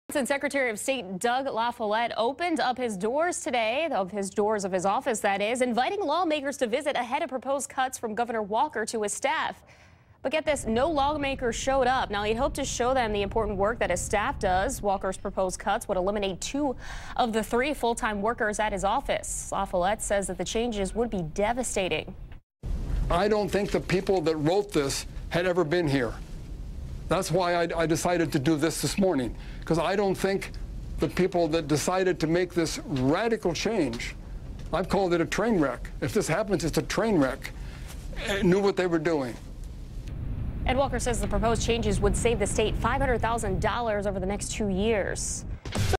Wisconsin Sec. of State Opposes Cuts 4pm 2-17-15
Secretary of State Doug La Follette says cuts to his office staff would be devastating.